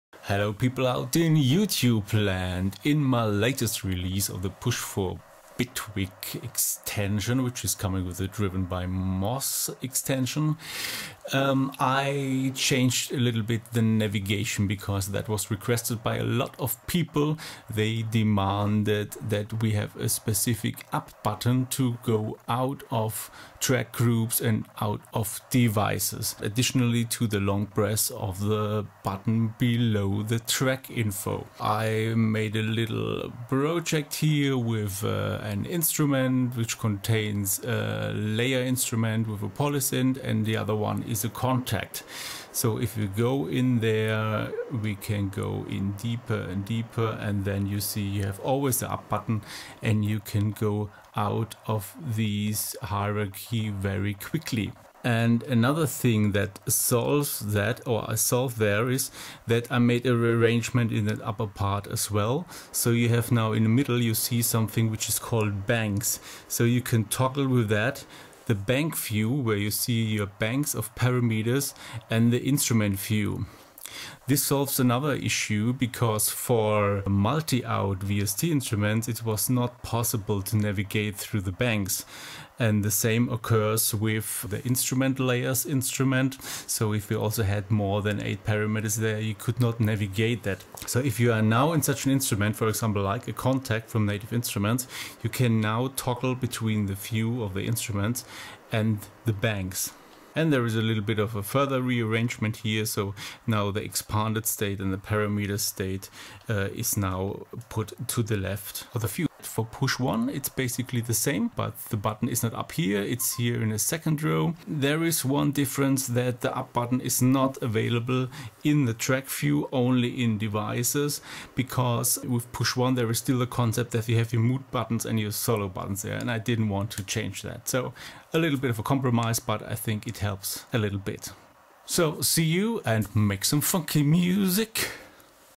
Push4Bitwig 9.22 - Navigation changes
I show the new features of track and device navigation coming with Push4Bitwig 9.22.